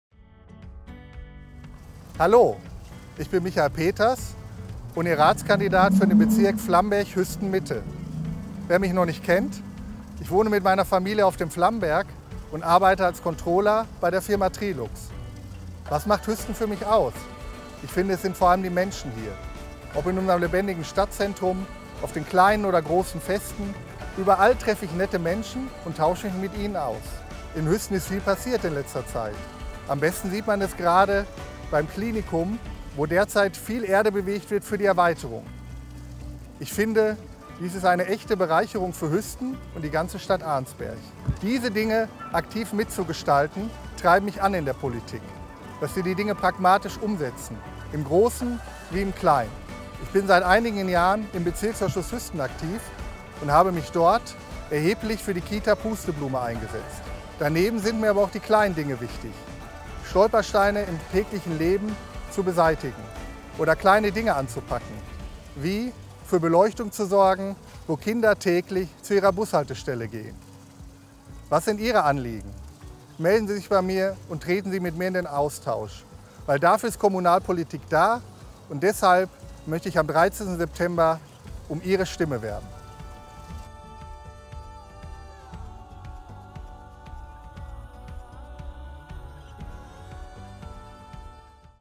Kommunalwahl 2020 - Michael Peters
Michael Peters kandidiert am 13. September für den Wahlbezirk "Hüsten-Mitte/ Flammberg".